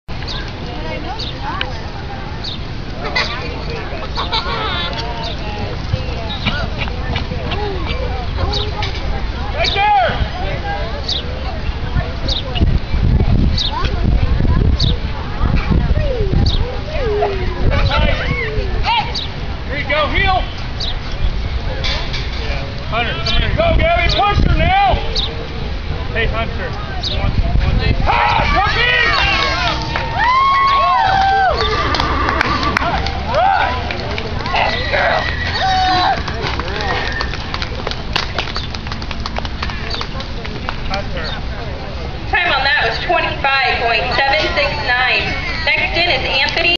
White County Fair 2013 - Gabby's run
One of my riding buddies. Our horses are attached at the hips its like, they are complete girlfriends, you'd think they're sisters, but they aint